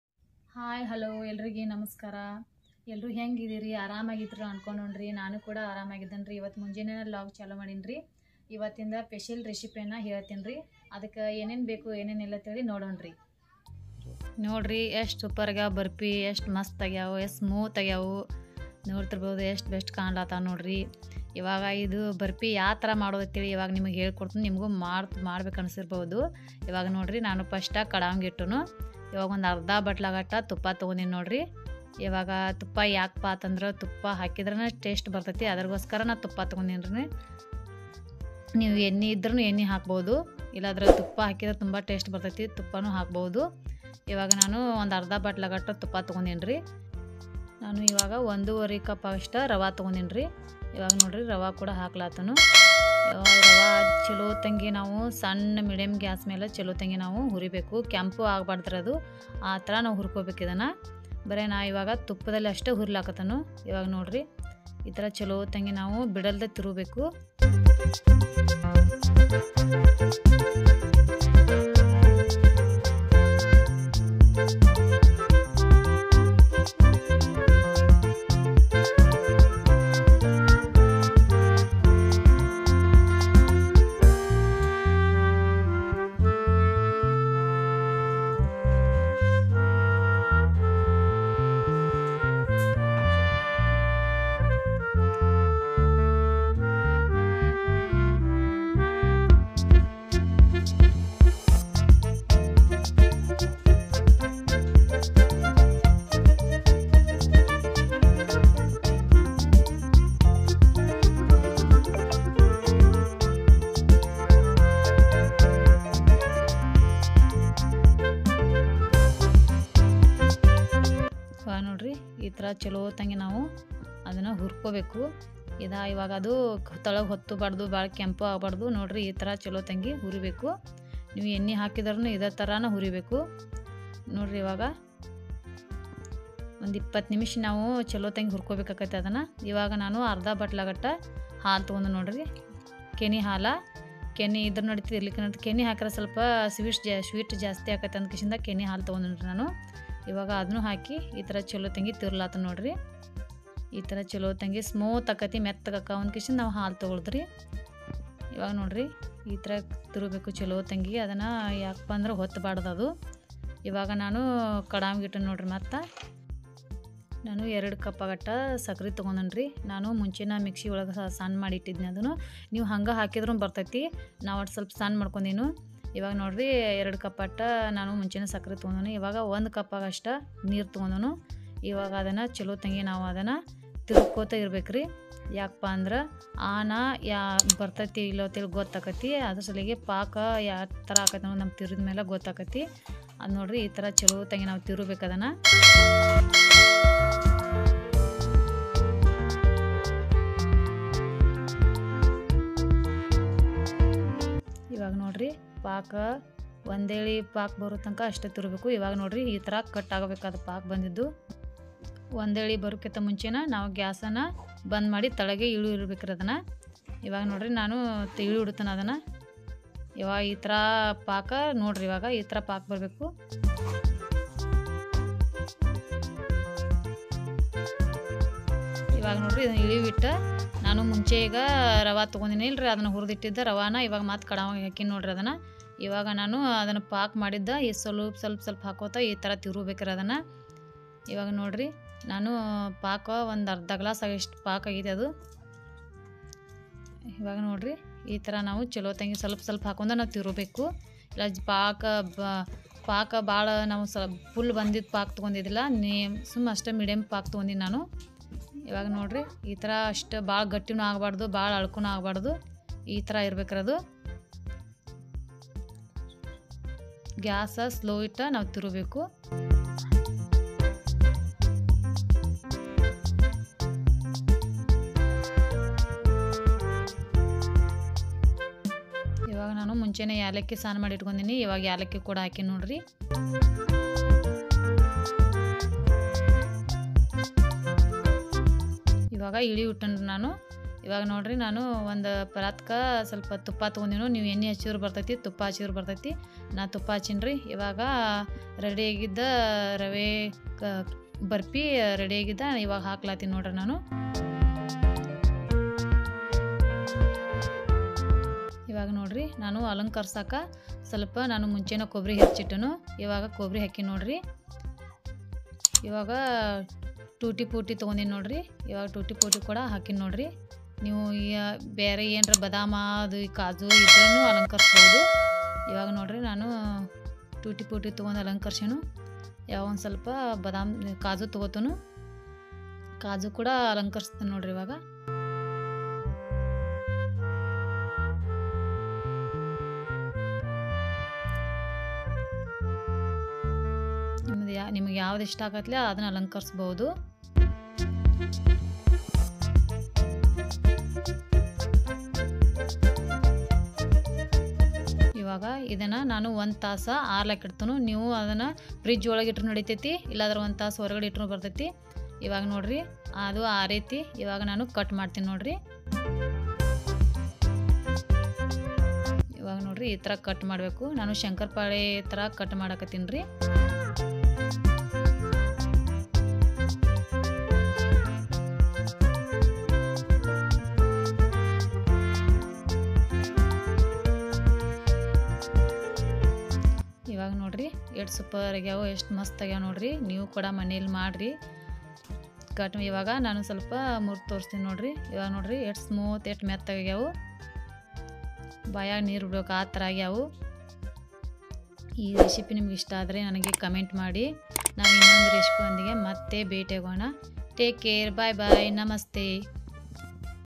ಗಣಪತಿ ಚತುರ್ಥಿ ಸ್ಪೆಷಲ್ ನೈವೇದ್ಯ ರವಾ ಬರ್ಪಿ ಸ್ವೀಟ್ ಬೇಕಾದಾಗ ಮಾಡಿ ತಿನ್ನಿ/
ರವಾ ಬರ್ಫಿ ಸ್ವೀಟ್ ಬೇಕಾದಾಗ ಮಾಡಿ ತಿನ್ನಿ